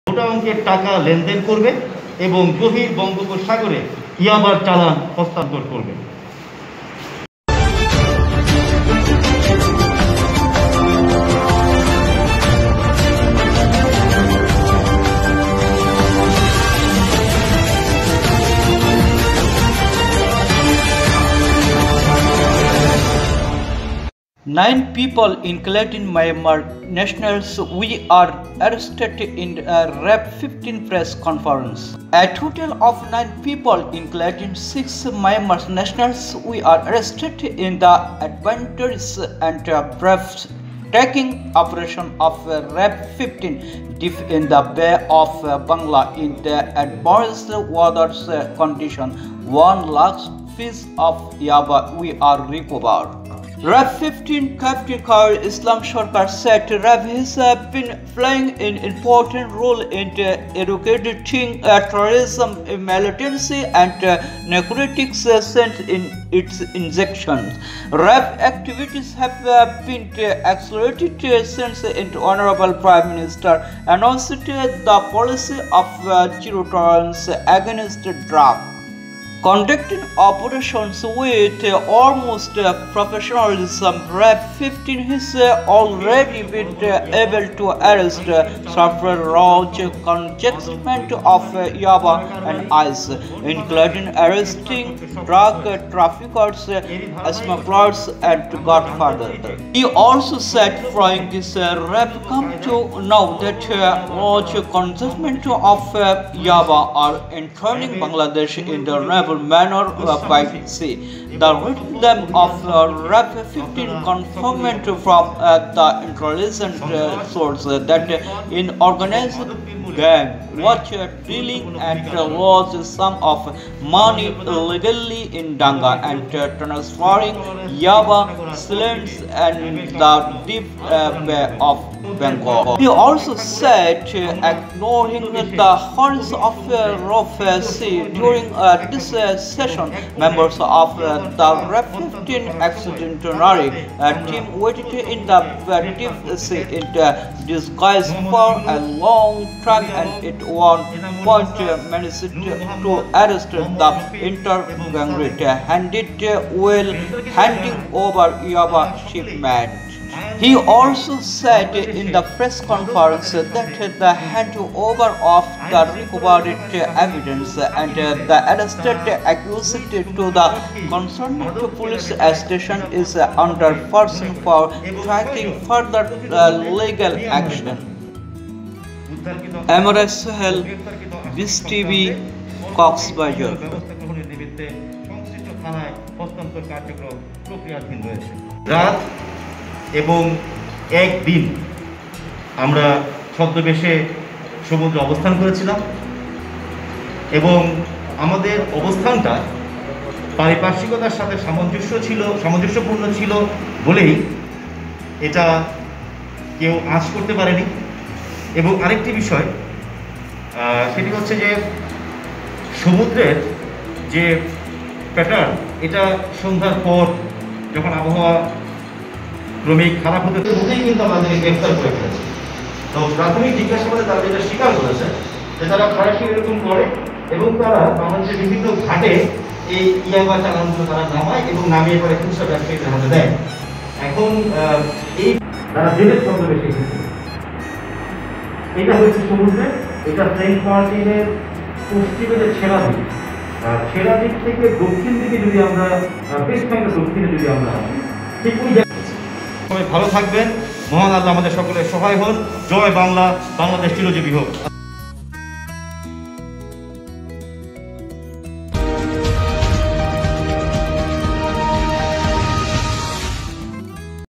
The press conference Ryab 15 captain Khairul Islam Sarkar said.
A total of 09 people including 06 Myanmar nationals were arrested in the adventurous and breath-taking operation of RAB-15 deep in the Bay of Bengal in adverse weather conditions, 01 lakh pieces of yaba were recovered.

. RAB has been playing an important role in eradicating terrorism, militan
cy and narcotics since its inception. RAB's activities have been accelerated since the Hon'ble Prime Minister announced the policy of "Zero Tolerance" against drugs.

Conducting operations with utmost professionalism, RAB-15 has already been able to arrest several large consignments of yaba and ice, including arresting drug traffickers, smugglers and godfathers.

Following this, RAB came to know that large consignments of yaba are entering Bangladesh in a novel manner by sea. The raid team of RAB-15 confirmed from the intelligence sources that an organized gang was dealing in large sums of money illegally in Danga and transferring Yaba challans in the deep Bay of Bengal.

Ignoring the horrors of rough seas during this season, members of the RAB expeditionary team waited in the deep sea in disguise for a long time and at one point managed to arrest the entire gang red-handed while handing over Yaba's shipment.

He also said in the press conference that the handover of the recovered evidence and the arrested accused to the concerned police station is under process for taking further legal action.